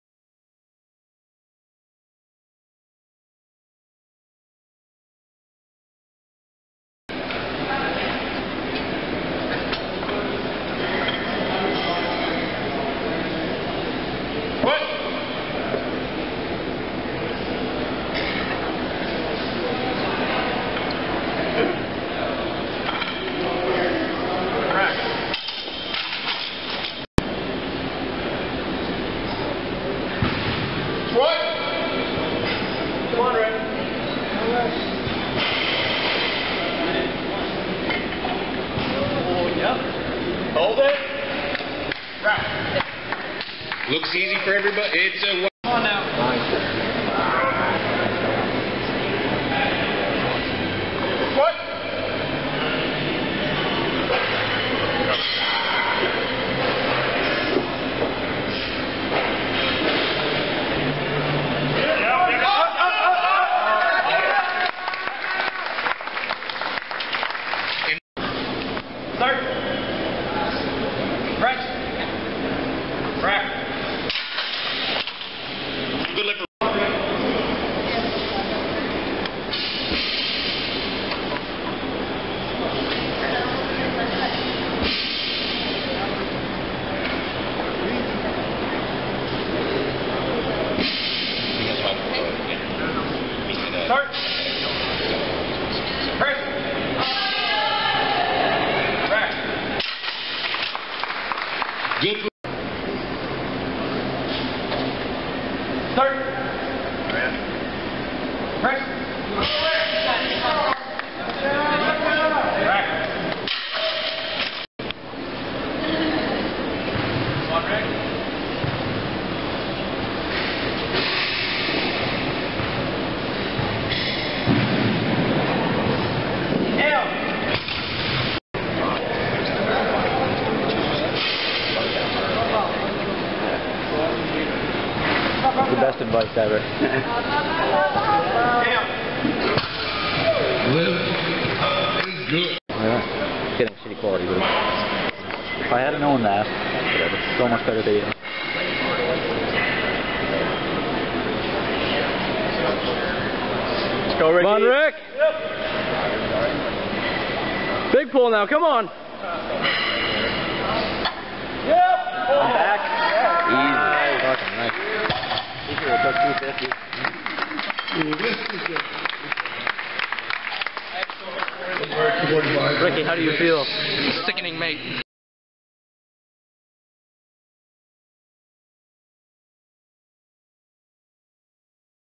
Ricki Porter NBPL (CPU) Provincials - 600/1322 @ 93kg class.
Awesome work man!